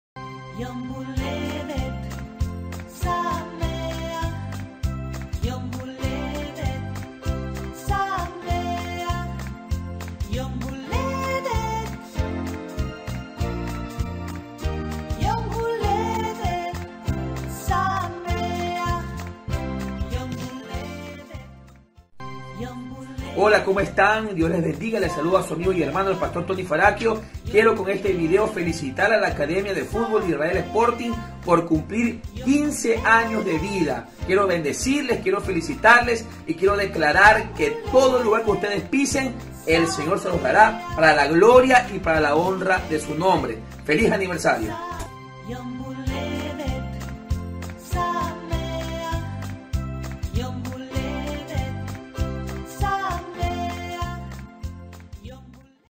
Aniversario #15 - Israel Sporting (Pastor. Tony Farachio, Guayaquil)
Gracias mi Pastor. Tony Farachio por sus Oraciones y poner en las manos del Eterno, el proyecto de nuestra Academia, y también gracias por sus palabras de felicitaciones a los Guerreros de David por nuestro #15 Aniversario. Shalom mi Pastor.